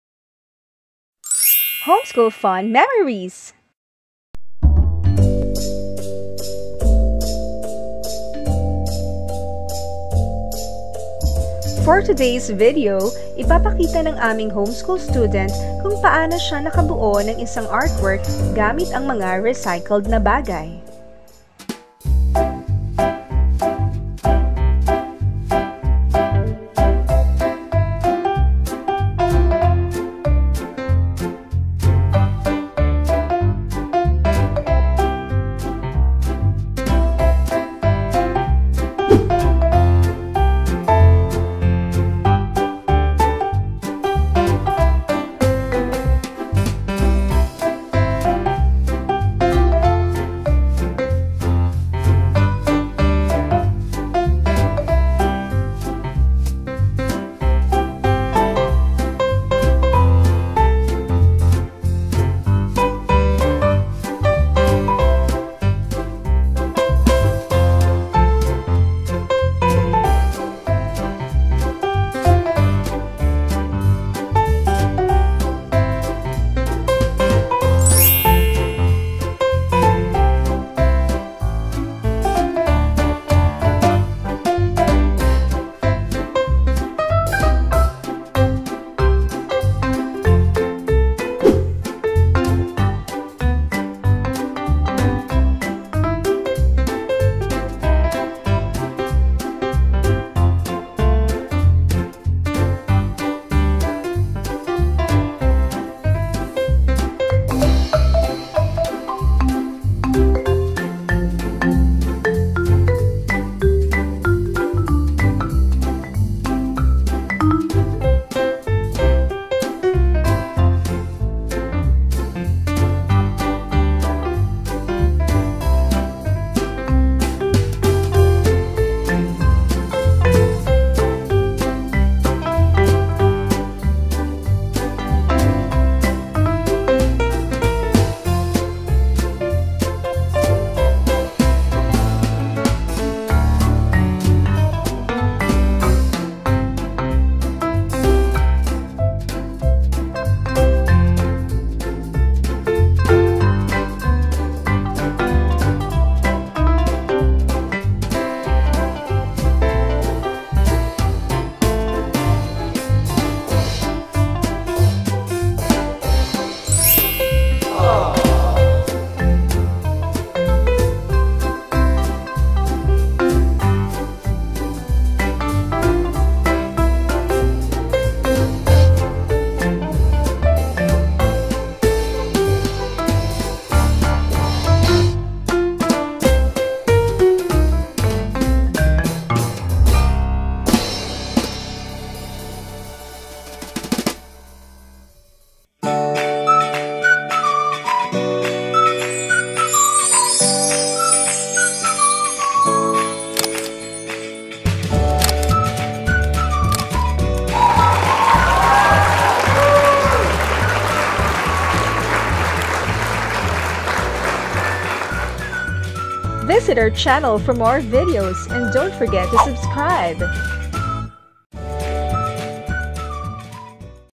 Turtle Craft, CD and Button Art, Likhang Sining Gamit ang Recycled Materials, Easy Crafts, Recycling
This video will show you how our homeschool student was able to create an artwork using recycled materials.

Ipapakita sa video na ito kung paano nakagawa ang aming homeschool student ng isang sining gamit ang mga patapong bagay o recycled materials.

Vector images are available at Vecteezy.com.